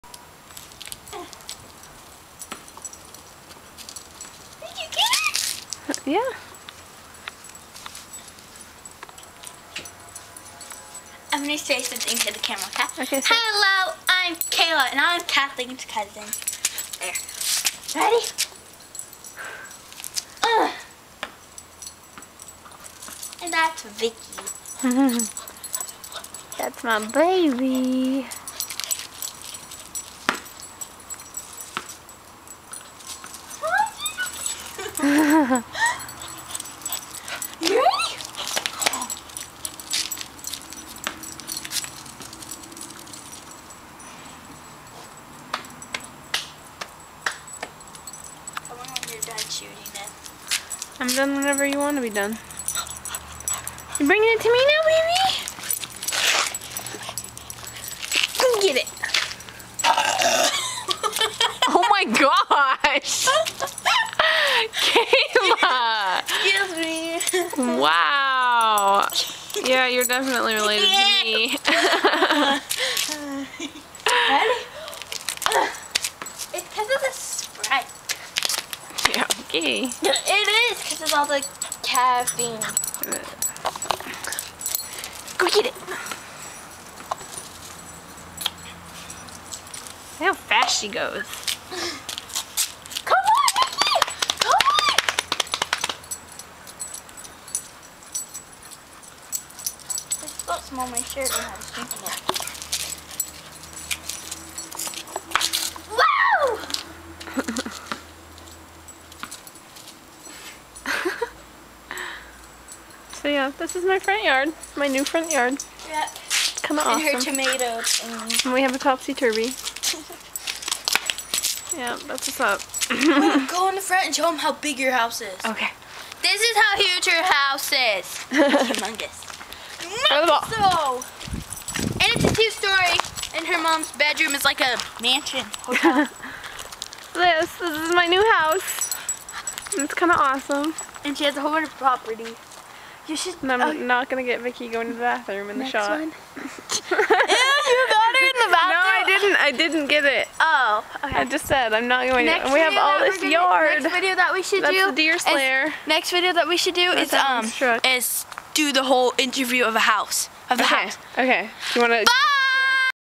Playing Fetch
my little cousin Kayla wanted to play fetch with my dog Vicki